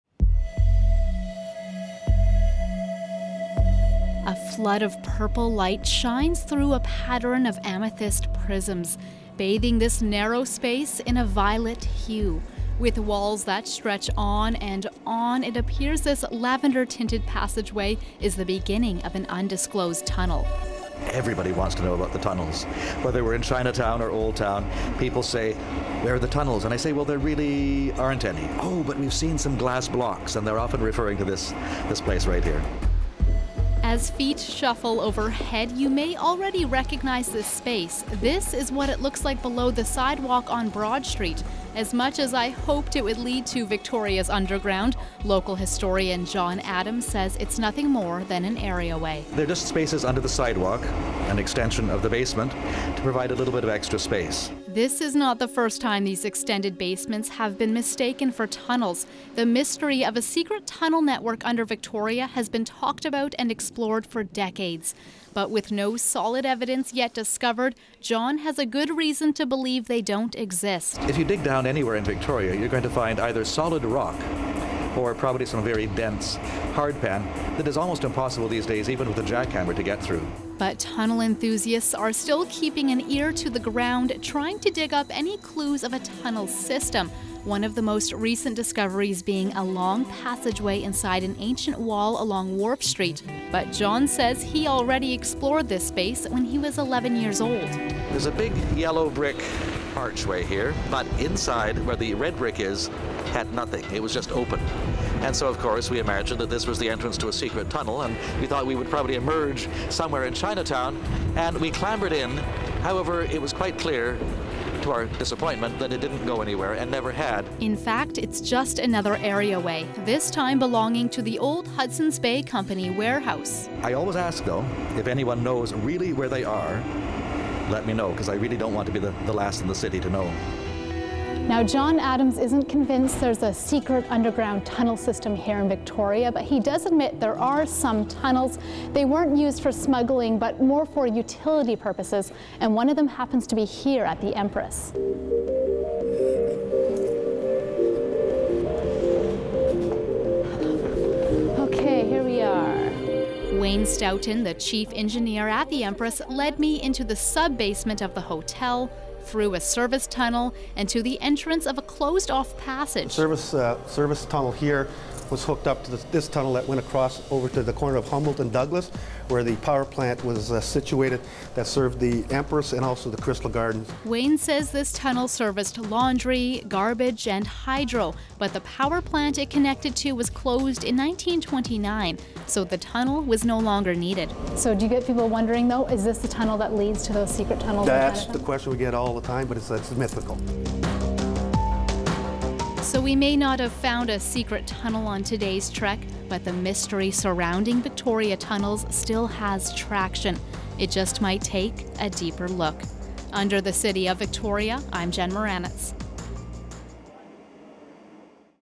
The Tunnels Under Broad Street - Shaw TV Victoria
Shaw TV's Jen Muranetz descends beneath street level to see if the stories of a secret tunnel network under Victoria are true.